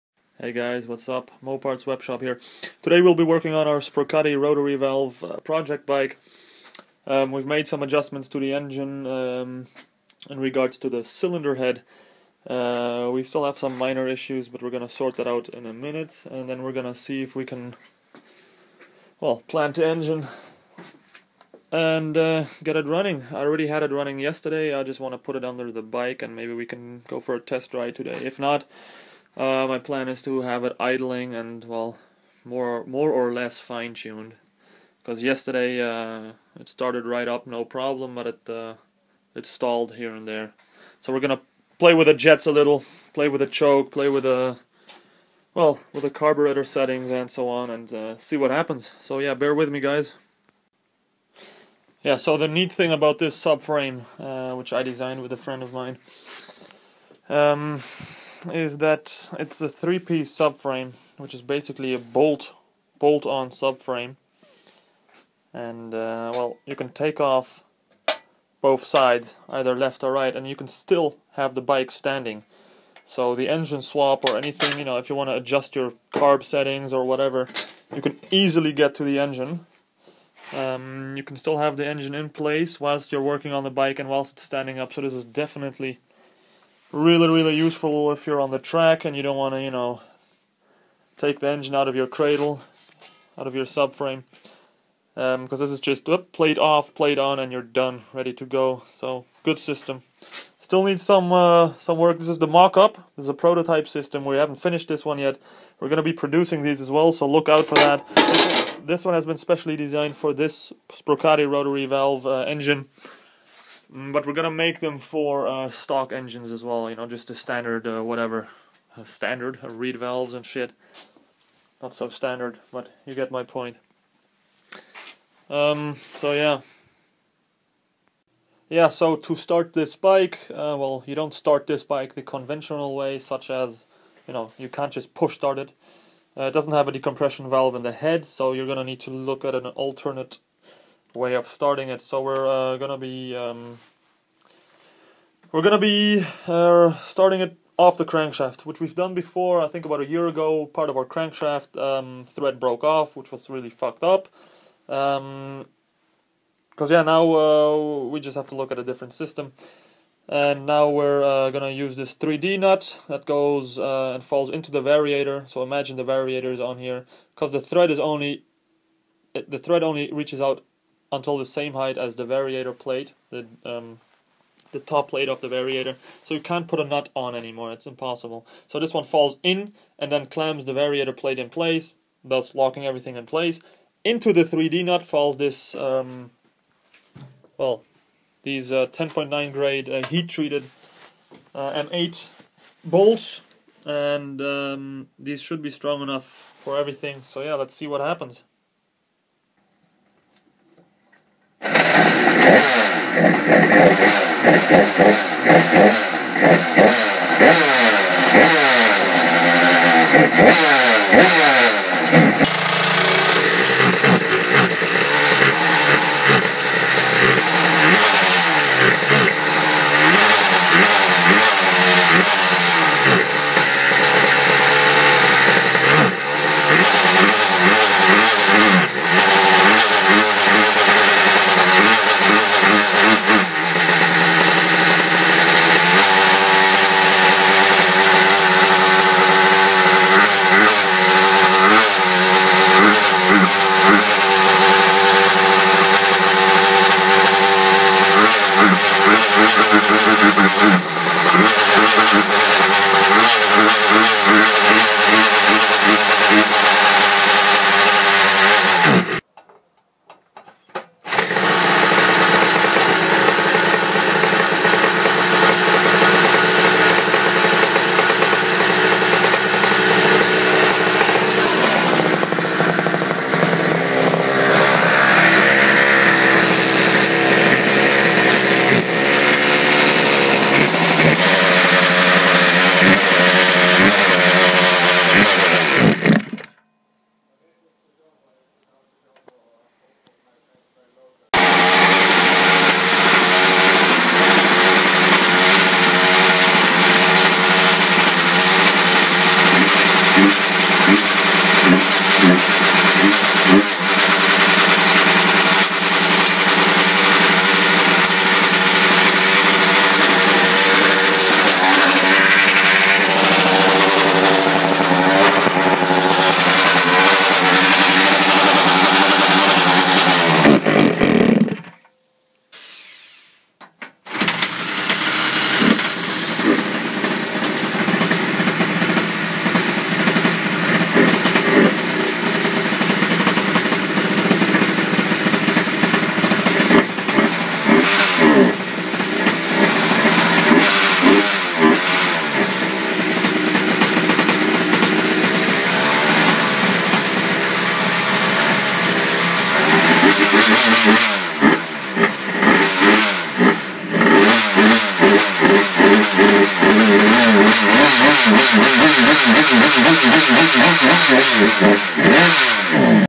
Sprocatti Rotary Valve (Disco Rotante) Vespa Piaggio
First tests of the Sprocatti Rotary Valve engine we have been working on. These are the first tests with its new Dell'orto 30 VHSH carburator.

This bike has a 50mm Parmakit cylinder kit (liquid cooled) and an HPI Internal Rotor System.

The bike has yet to be fine-tuned. We will be working on that the coming time.

We are also working on a hybrid transmission system. By hybrid we basically mean that the clutch is one off a scooter (Minarelli). We will be using a stock transmission (pulley transmission) that will be adjusted to work with the shaft of a scooter transmission. Advantages of this system are that it is stronger and can better handle the power created by the engine. The stock Vespa/Piaggio variated transmission system cannot handle and fully transfer the power of this engine.

Our goal is to have the most powerful Vespa/Piaggio bike, for the automatic class (variated).

2 September 2013